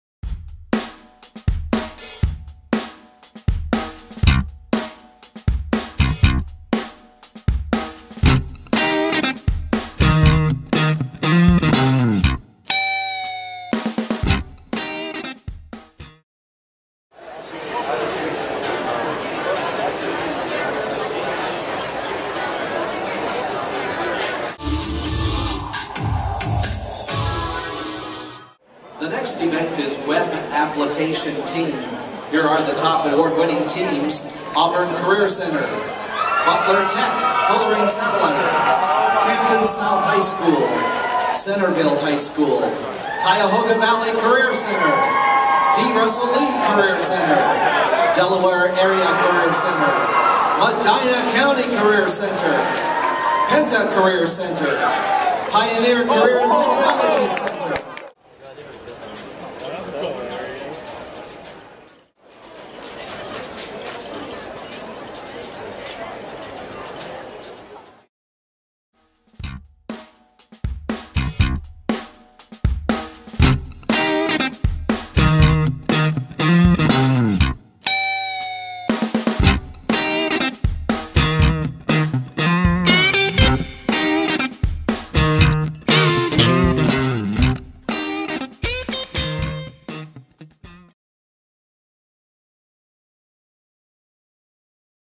2011 Business Professionals of America Awards
Chistopher Adams and Jacob Mullins placed  top 10, Ohio in "Web Application"
 at the 2011 Business Professionals of America, state level competition.